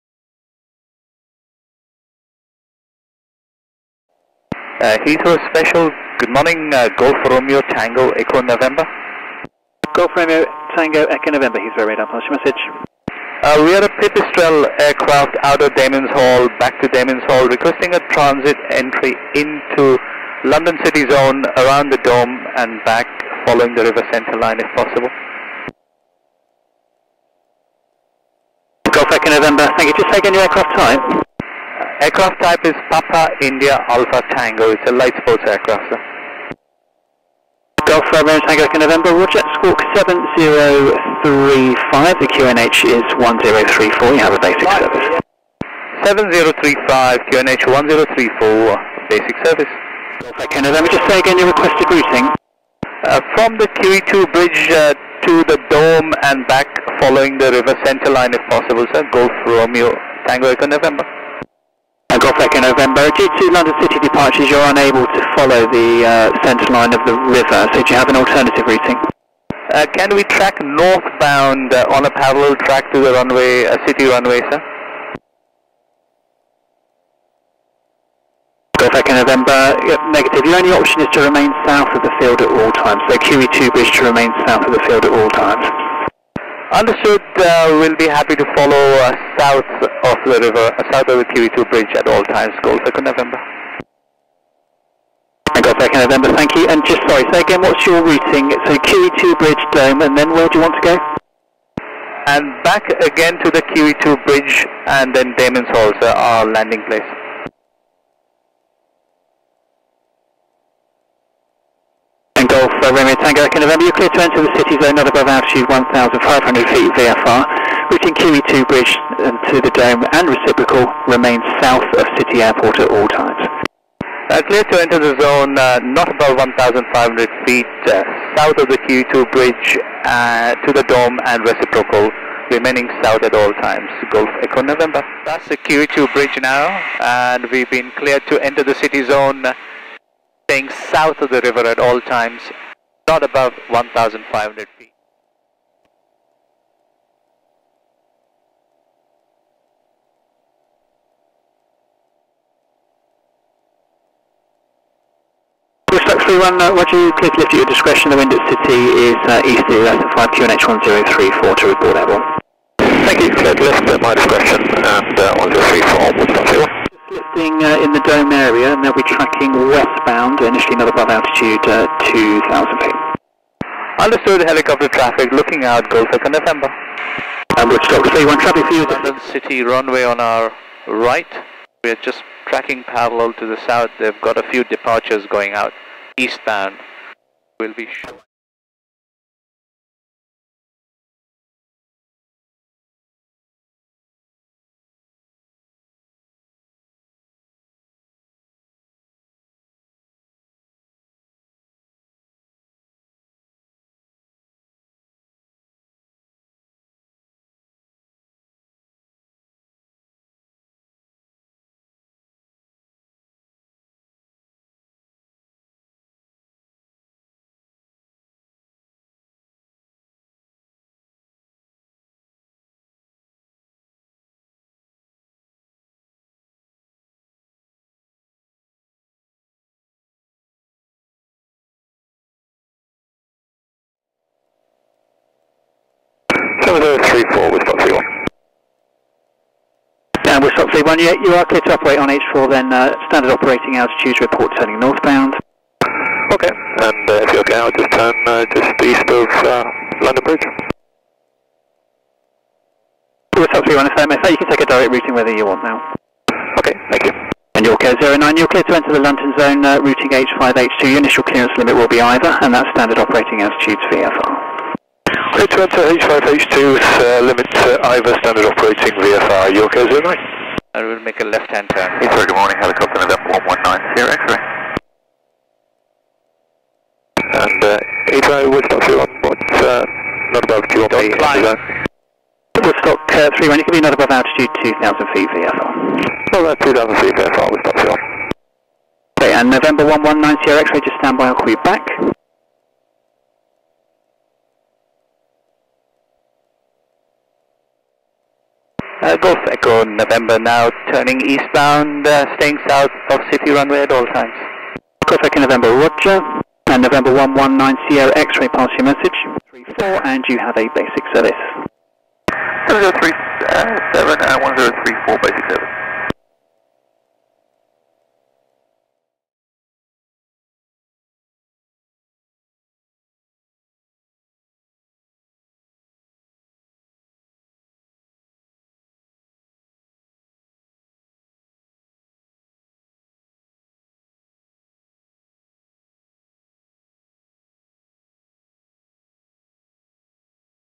London City Zone Transit SD 480p
Training flight for use of RT to fly into and across control zone.
Plan your flight with alternate routes, prepare and rehearse your radio script. Practice, practice, practice.
If you listen carefully, the Rt is relaxed, colloquial and friendly.
The Heathrow ATC is offering you a service !
You can negotiate a route if your preferred route is not available.